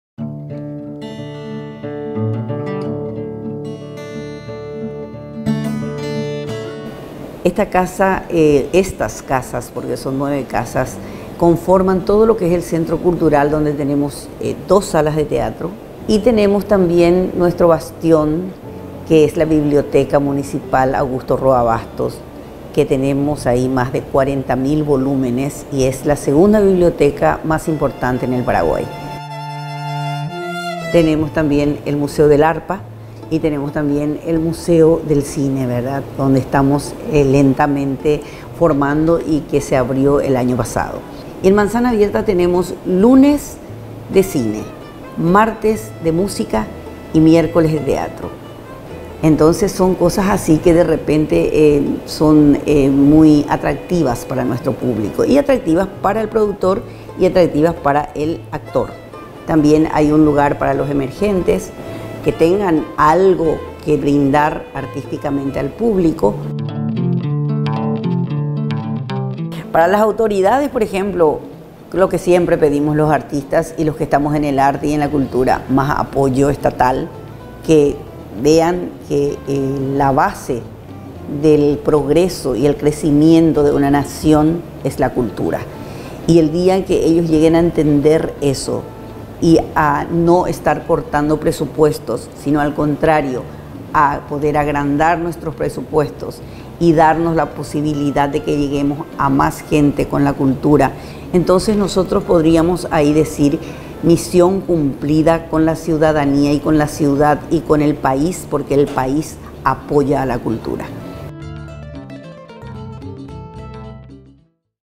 Manzana de la Rivera abre ciclo artístico para disfrutar el microcentro de Asunción
El ciclo cultural Manzana Abierta, por segundo año consecutivo abrió sus puertas con el objetivo de promover el arte junto a productos nacionales. La programación artística propone desde producciones audiovisuales hasta obras teatrales.